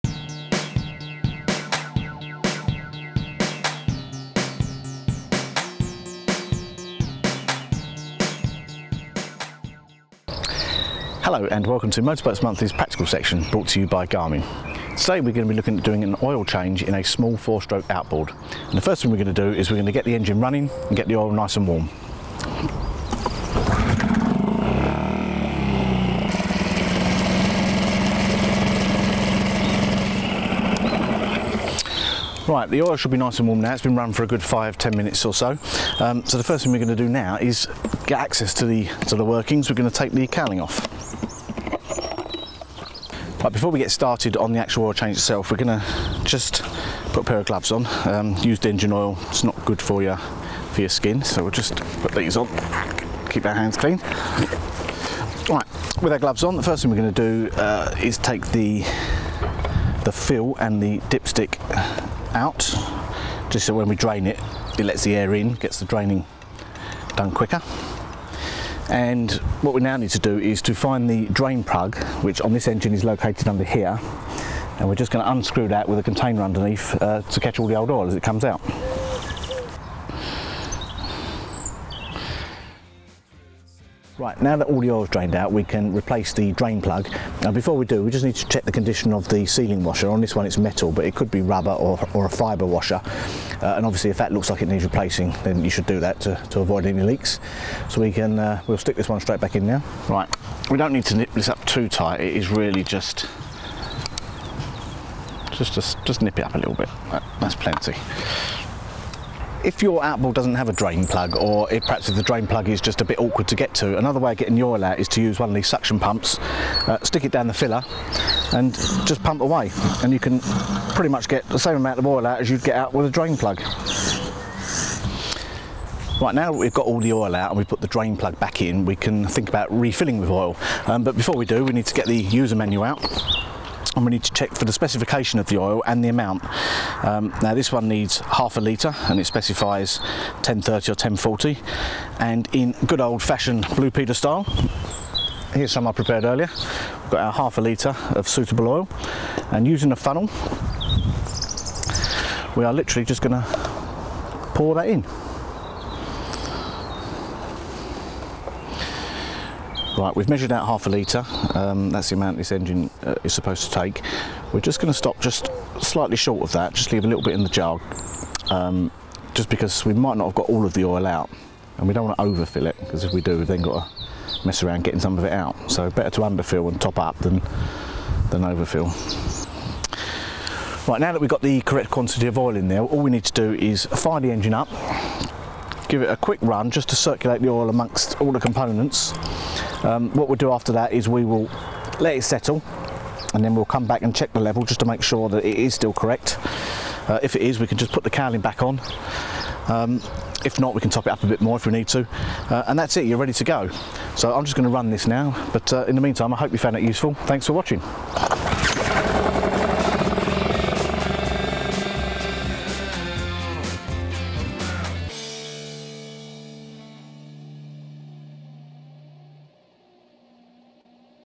MBM's practical boating: changing your outboard engine oil
In this step-by-step guide, Neale Byart talks you through how to change the engine oil in your tender outboard